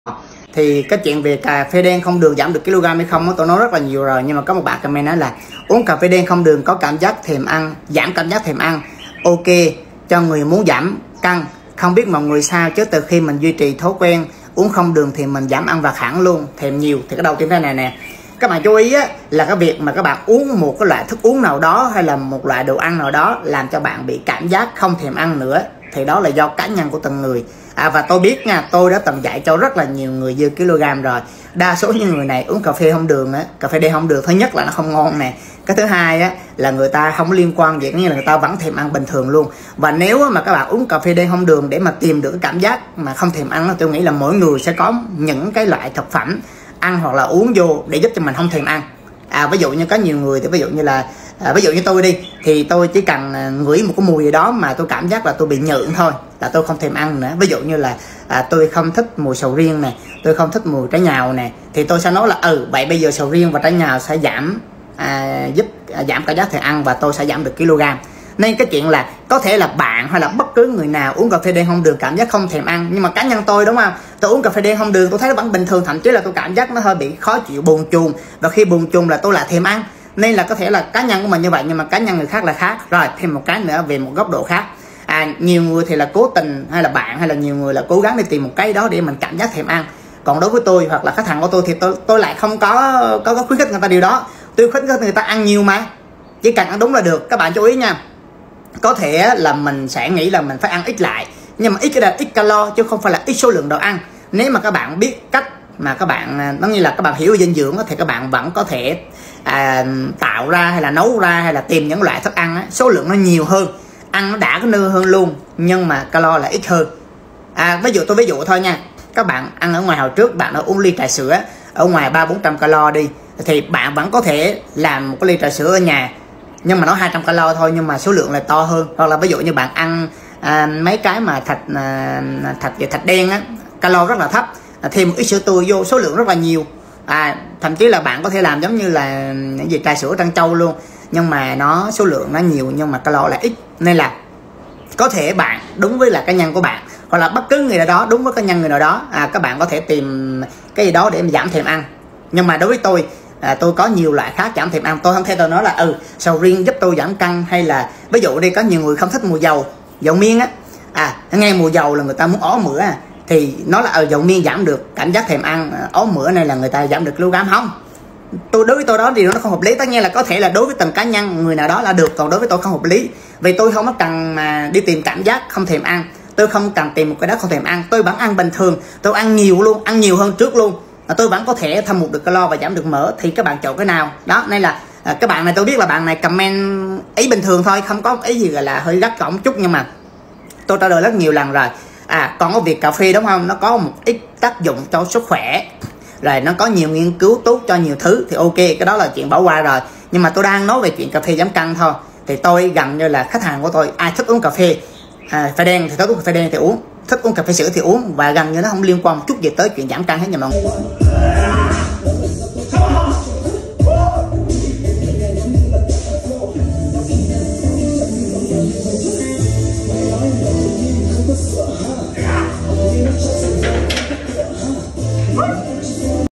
Cà phê đen không đường có thực sự giúp giảm cân?
Cà phê đen không đường có thực sự giúp giảm cân? cafeden giảmcân thèmăn thựcphẩm dinhduong gym workout dohoanggym Tôi đã nói rất nhiều về việc uống cà phê đen không đường có thể giúp giảm cân. Một người bạn đã bình luận rằng uống cà phê đen không đường có thể làm giảm cảm giác thèm ăn, điều này có thể hữu ích cho những người muốn giảm cân. Tuy nhiên, tôi nhận thấy rằng điều này phụ thuộc vào cá nhân mỗi người. Một số người có thể cảm thấy không thèm ăn khi uống một số thức uống hoặc thức ăn nhất định, nhưng với tôi thì không phải như vậy. Tôi vẫn ăn bình thường và thậm chí còn ăn nhiều hơn trước đây. Thay vì tìm cách để không thèm ăn, tôi khuyên mọi người nên tập trung vào việc ăn đúng cách, tức là ăn nhiều nhưng với lượng calo ít hơn.